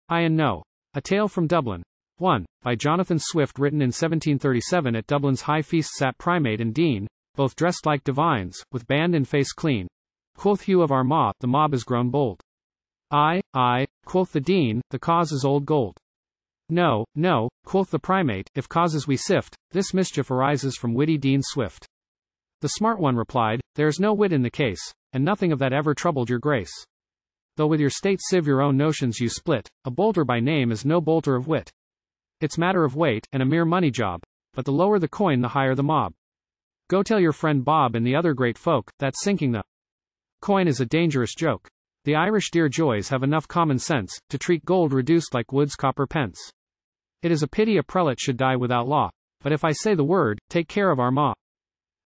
Ay And No A Tale From Dublin.1 by Jonathan Swift
At Dublin's high feast sat Primate and Dean,

    Both dress'd like divines, with band and face clean:

    Quoth Hugh of Armagh, "The mob is grown bold."

    "Ay, ay," quoth the Dean, "the cause is old gold."

    "No, no," quoth the Primate, "if causes we sift,

    This mischief arises from witty Dean Swift."

    The smart one replied, "There's no wit in the case;

    And nothing of that ever troubled your grace.

    Though with your state sieve your own notions you split,

    A Boulter by name is no bolter of wit.

    It's matter of weight, and a mere money job;

    But the lower the coin the higher the mob.

    Go tell your friend Bob and the other great folk,

    That sinking the coin is a dangerous joke.

    The Irish dear joys have enough common sense,

    To treat gold reduced like Wood's copper pence.

    It is a pity a prelate should die without law;

    But if I say the word - take care of Armagh!"